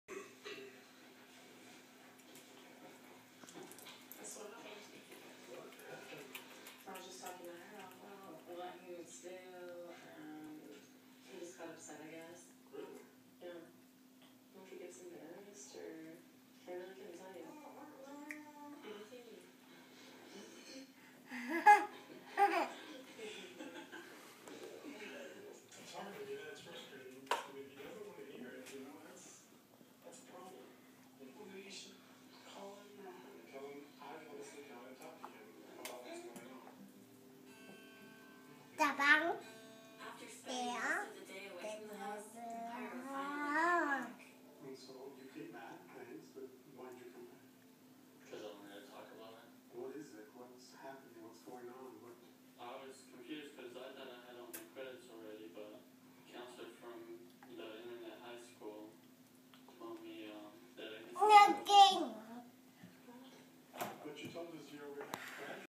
Rylan eating a lemon
Rylan decided he wanted to eat the lemon that was leftover.